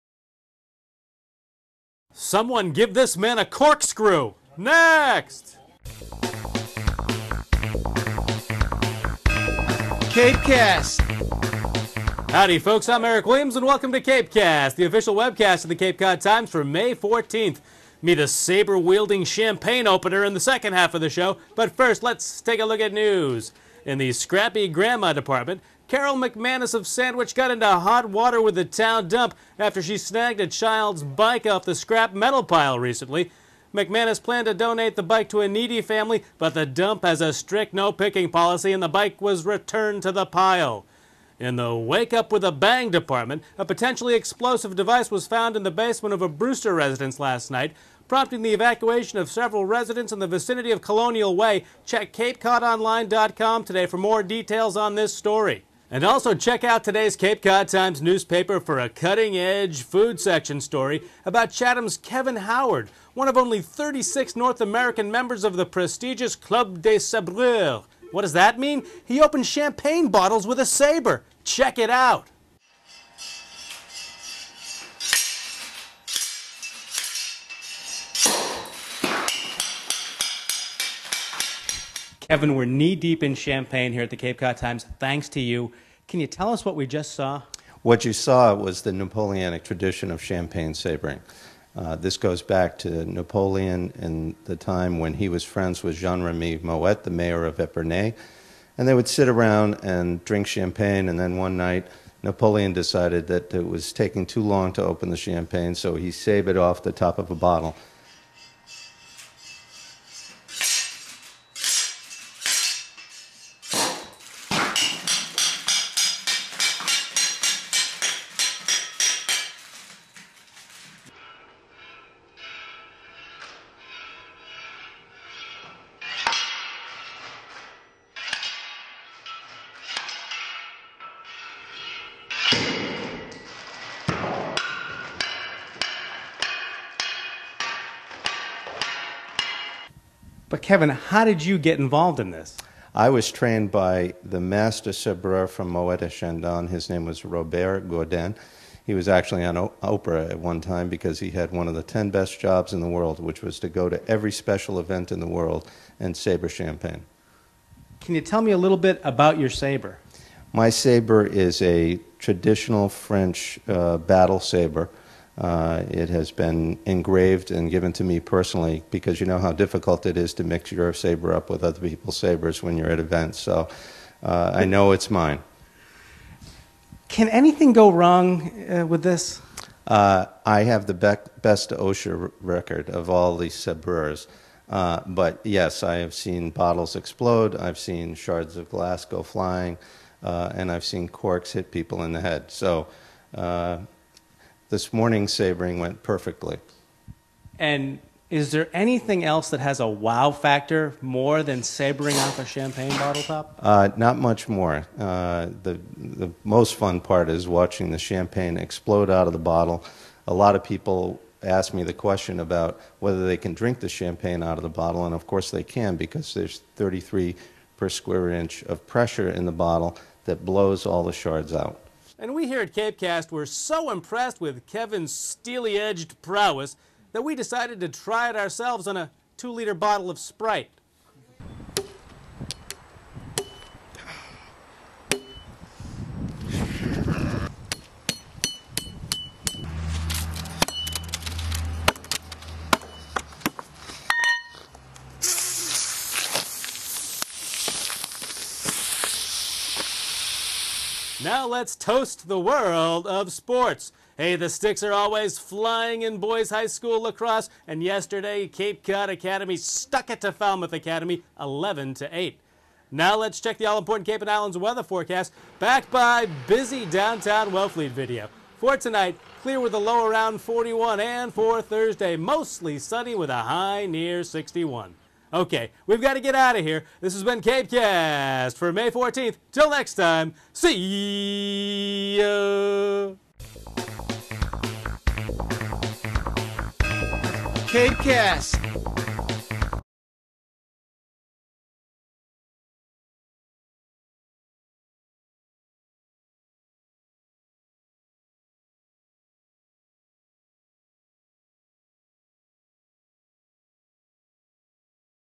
Watch as champagne is uncorked using a saber!
On today's CapeCast: Meet Kevin Howard of Chatham, saber-wielding champagne-opener and see the froth fly--plus Sandwich dump-picker in hot water.